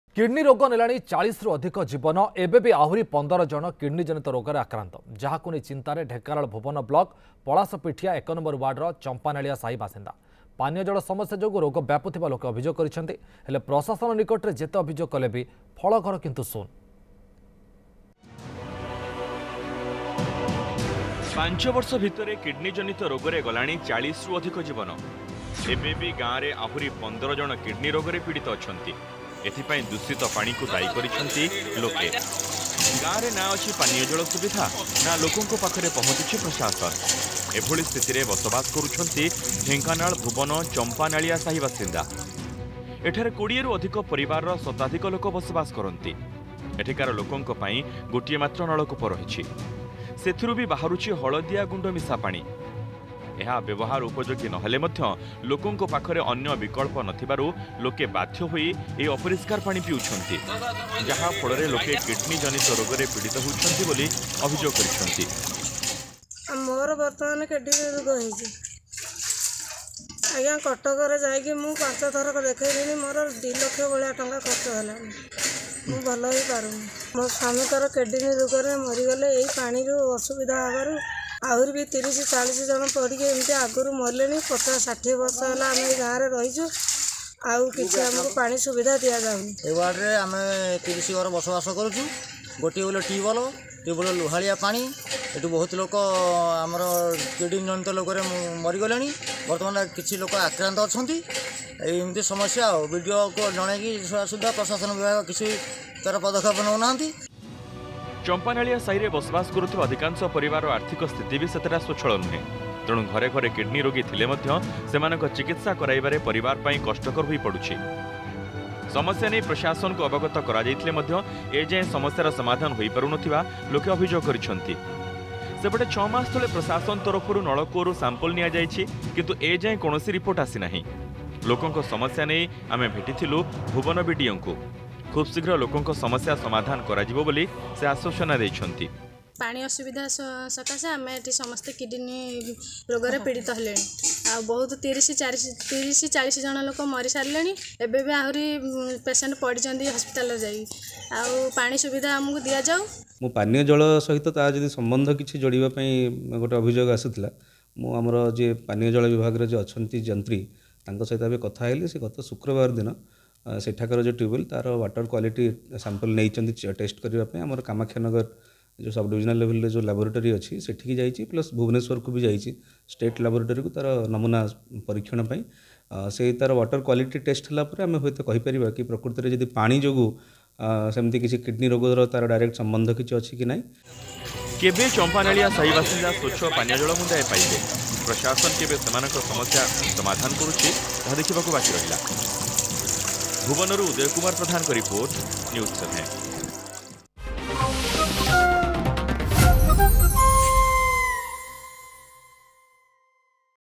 କିଡ଼ନୀ ରୋଗୀଙ୍କ ଗାଁ ଚମ୍ପାନାଳିଆ, ୫ବର୍ଷରେ କିଡ଼ନୀ ରୋଗରେ ଗଲାଣି ୪୦ରୁ ଅଧିକ ଜୀବନ| Village of kidney patients
କିଡ଼ନୀ ରୋଗୀଙ୍କ ଗାଁ ଚମ୍ପାନାଳିଆ, ୫ ବର୍ଷରେ କିଡ଼ନୀ ରୋଗରେ ଗଲାଣି ୪୦ରୁ ଅଧିକ ଜୀବନ | Champanalia: Village of kidney patients